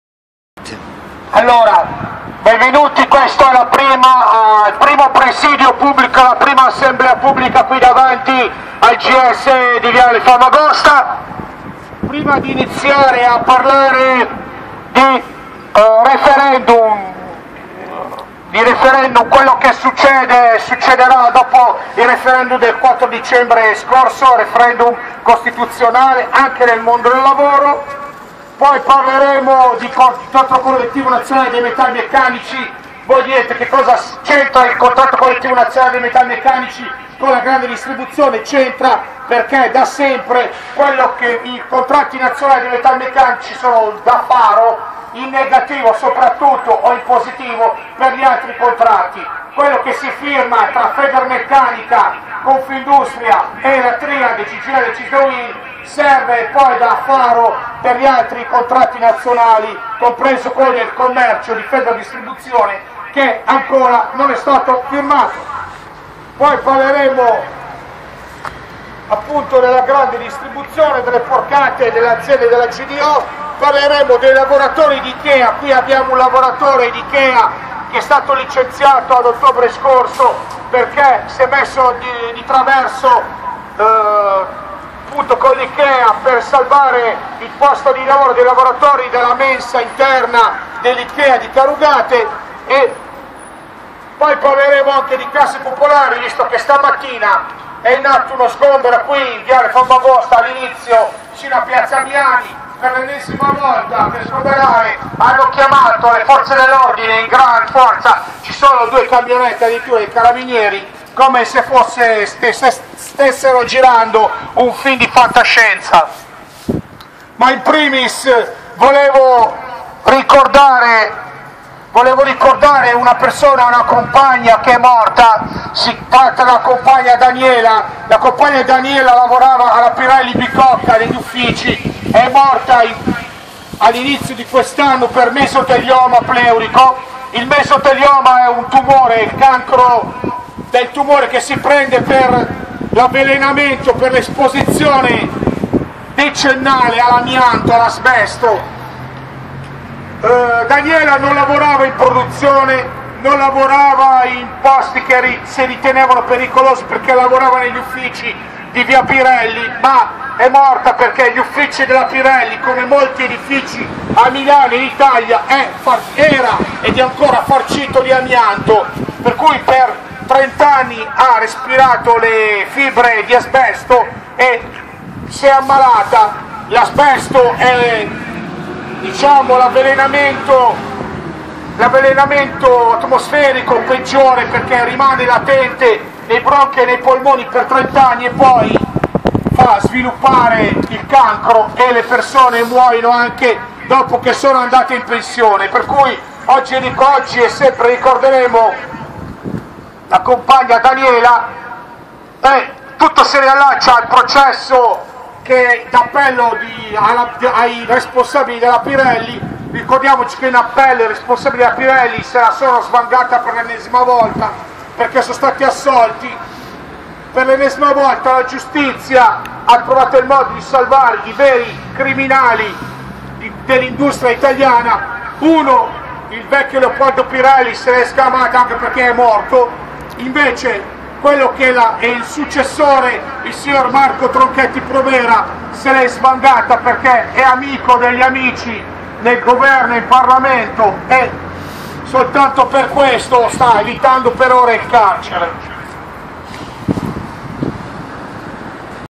Quartiere Barona:diario di uno sgombero programmato-11 gennaio 2017-2 parte
In questo video racconto il senso del presidio di oggi in Famagosta, e le porcate dei padroni:su tutte la morte della Compagna Daniela di mesotelioma per colpa dei criminali della Pirelli che per ora hanno evitato la galera.
Inoltre abbiamo introdotto lo sgombero dello stabile di Viale Famagosta che, ieri mattina,  ha catalizzato l' attenzione di tutto il quartiere.